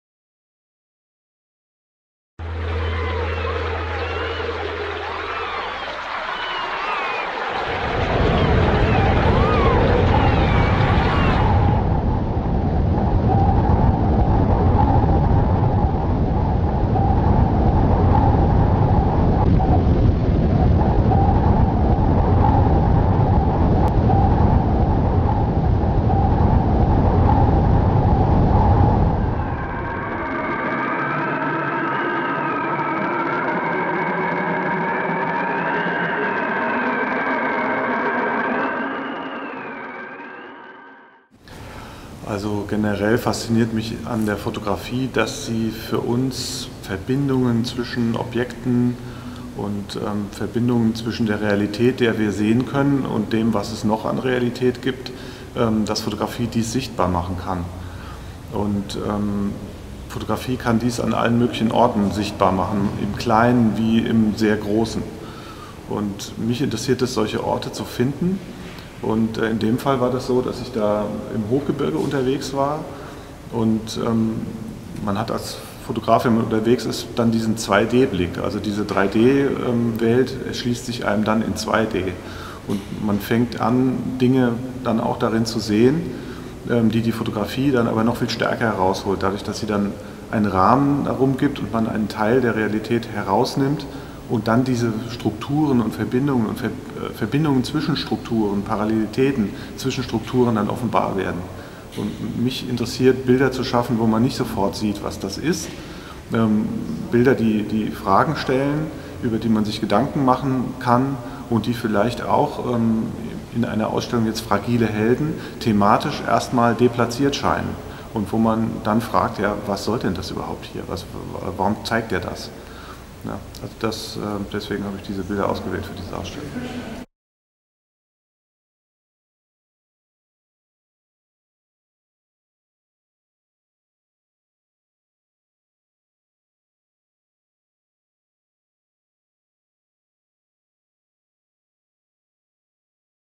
Kunstverein Familie Montez | Fragile Helden | Interview mit Peter Braunholz | 2012
51 | Kunstverein Familie Montez | Fragile Helden | Interview mit Peter Braunholz | Farbe | 2:23 min. Gruppenausstellung: Fragile Helden. 19. November - 27. Dezember 2011. Direktor: Mirek Macke. Peter Braunholz zeigt “Massives”. | Das Ausstellungsprojekt wurde unterstützt vom Kulturamt der Stadt Frankfurt am Main.